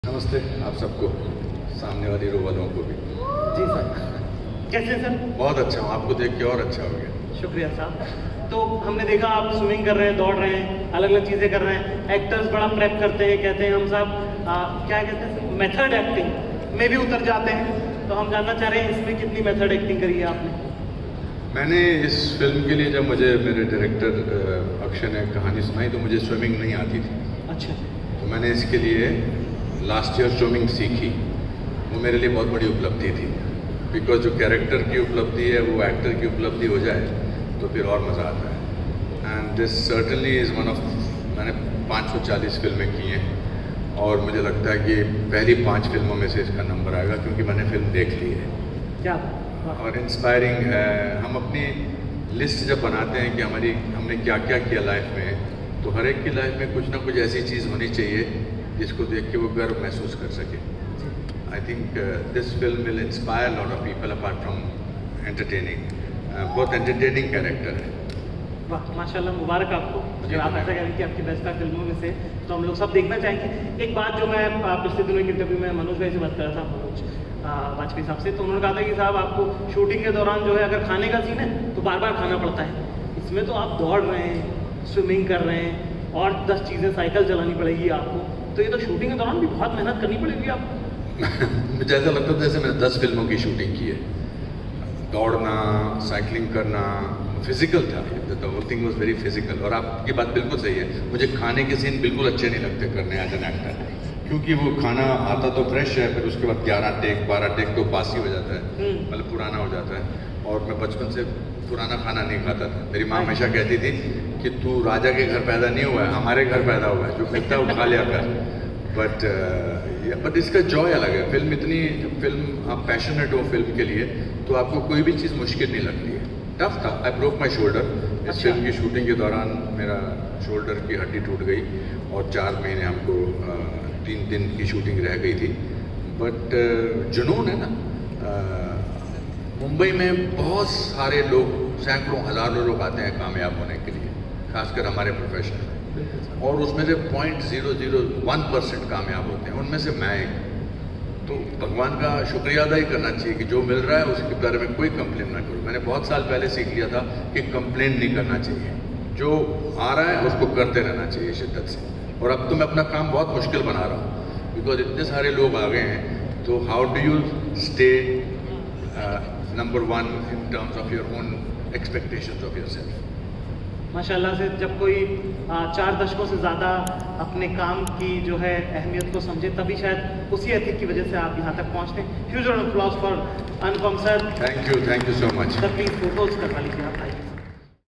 Anupam Kher ने अपनी माँ Dulari के बारे में कही ऐसी बात, हंस पड़ेंगे आप | Next On Netflix | Vijay 69
Anupam Kher shared his childhood memories at the next on Netflix event. He also shared why Vijay 69 is very special for him